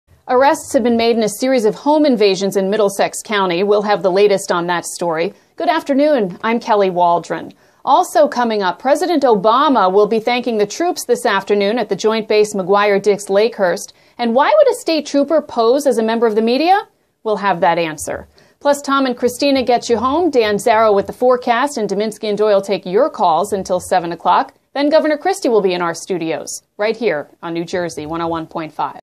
Afternoon news headlines Monday, December 15
It's a busy afternoon ride home as President Obama thanks U.S. troops and their families for their service in Afghanistan during an appearance at Joint Base McGuire-Dix-Lakehurst and details are released about arrests in a series of home invasions in Middlesex County.